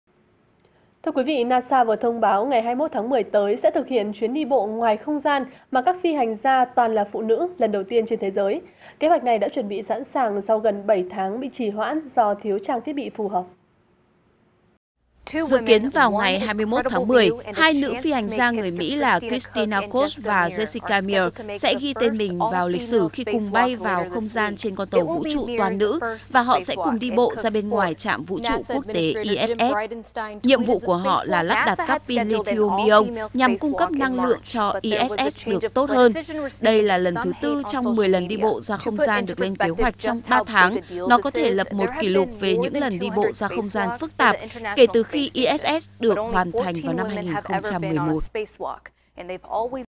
Nhóm nữ phi hành gia đầu tiên đi bộ ngoài không gian
NASA vừa thông báo, ngày 21/10 tới sẽ thực hiện chuyến đi bộ ngoài không gian toàn là phụ nữ lần đầu tiên trên thế giới. Kế hoạch này đã chuẩn bị sẵn sàng sau gần bảy tháng bị trì hoãn do thiếu trang thiết bị phù hợp.